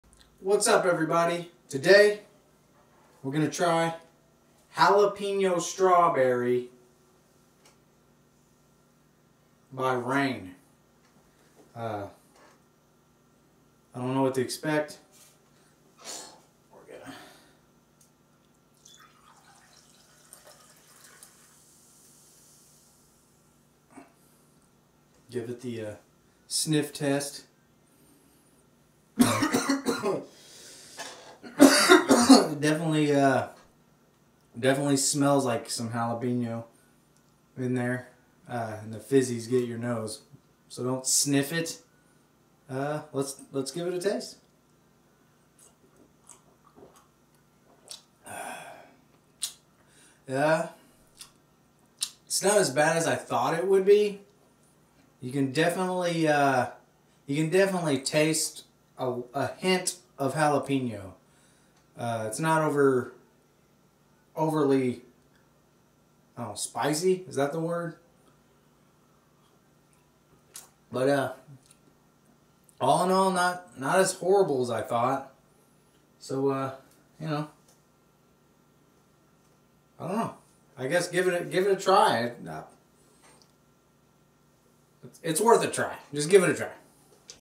Reign Inferno Jalapeno Strawberry
Today I do a taste test of Reign Inferno Jalapeno Strawberry.